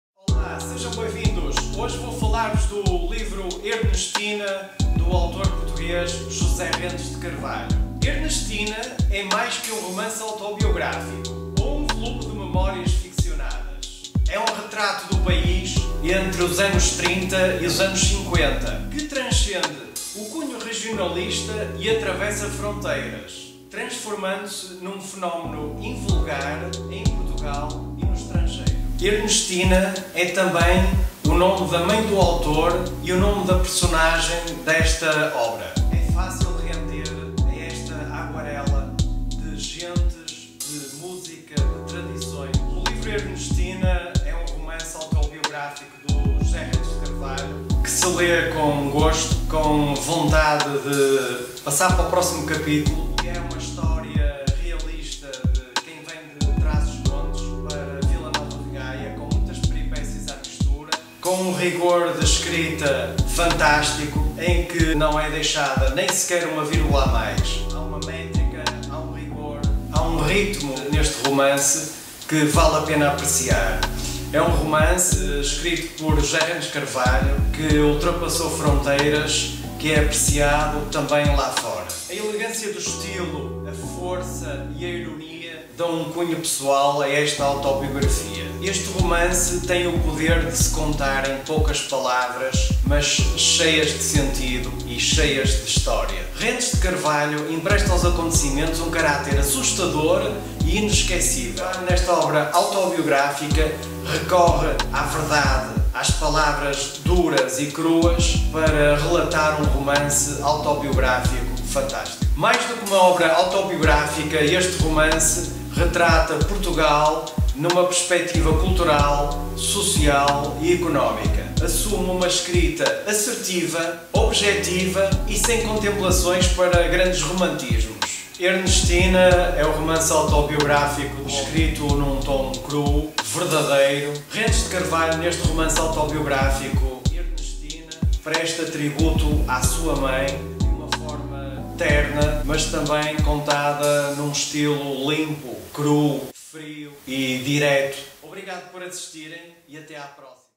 ERNESTINA de J. Rentes de Carvalho da série Livros #02
Romance autobiográfico e volume de memórias de famílias ficcionadas. Fresco de Trás-os-Montes, dos anos 30 aos anos 50. Obra que transcende o relato regionalista e que transpôs fronteiras, transformando-se num fenómeno editorial na Holanda.
Ernestina é também o nome da mãe do autor e da intrépida protagonista deste livro. Sobre ela J. Rentes de Carvalho disse: «Mãe de um só filho, a sua vida, que foi de uma tristeza, amargura e terrível solidão, dava um livro.
Escrevi-lho eu.
E a sua morte quebra o último elo carnal que me ligava à terra onde nasci. Felizmente são ainda muitos os laços que a ela me prendem.»

Ernestina
de José Rentes de Carvalho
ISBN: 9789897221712
Idioma: Português
Tipo de Produto: Livro
Classificação Temática: Livros em Português - Literatura - Romance

Romance autobiografico de J. Rentes de Carvalho

SINOPSE
Plano Nacional de Leitura
Livro recomendado para a Formação de Adultos, como sugestão de leitura.